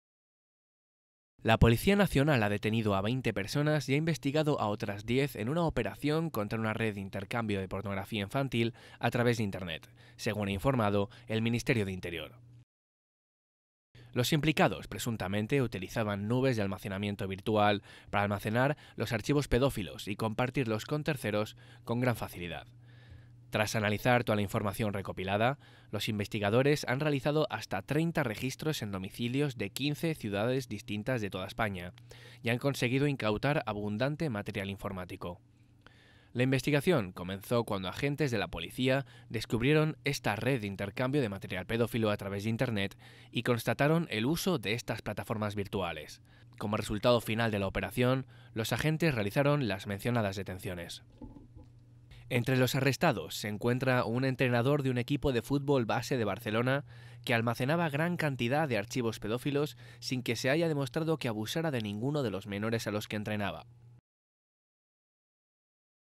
Detienen a 20 en operación contra intercambio de pornografía infantil
La Policía Nacional ha detenido a 20 personas y ha investigado a otras 10 en una operación contra una red de intercambio de pornografía infantil a través de Internet, según ha informado el Ministerio del Interior.